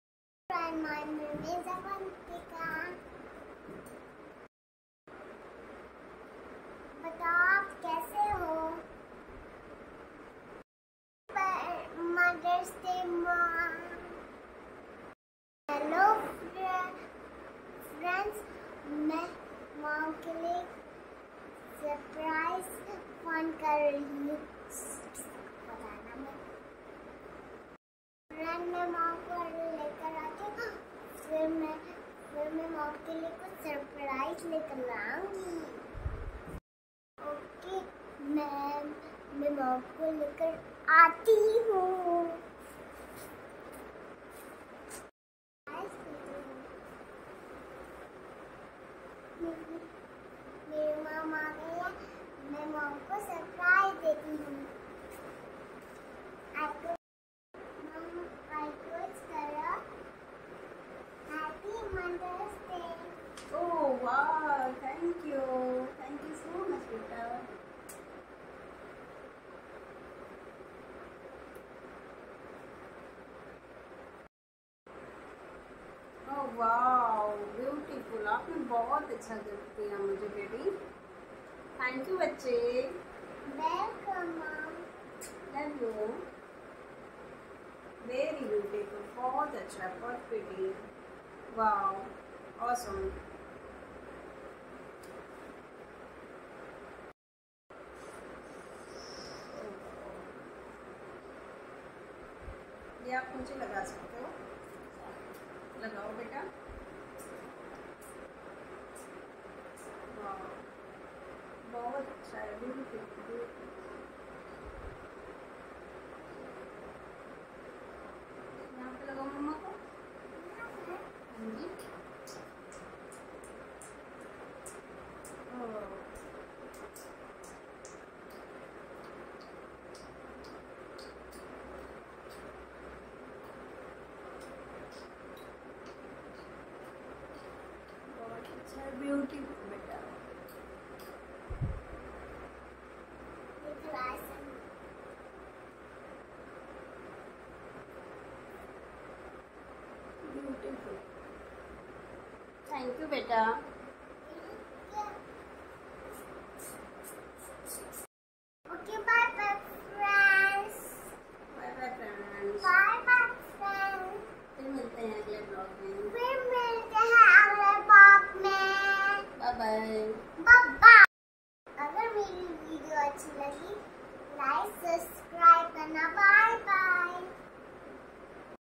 mother's day special surprise vlog ❤️❤️🌹🌹🥰🥰💐💐🎁🎁# मैंने अपनी मॉम को मदर्स डे पर सरप्राइस किया#
# मैंने अपनी मॉम को मदर्स डे पर सरप्राइस किया# (Happy mother's day dear mom ❤️)